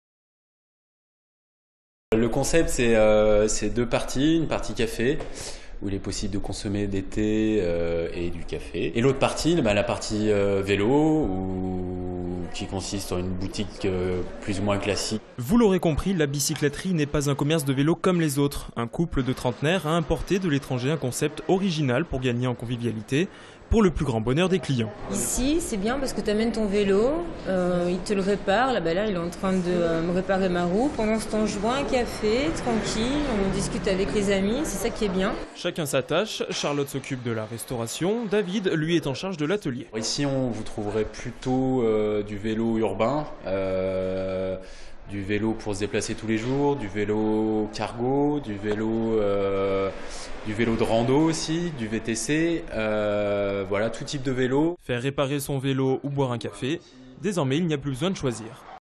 La Bicycletterie: un commerce de vélo pas comme les autres!
Sites webs/blogs, copiez cette vidéo avec le bouton "Intégrer" !
 Gros plan sur La bicycletterie, un concept store qui mêle atelier de réparation, magasin de bicyclettes et bar.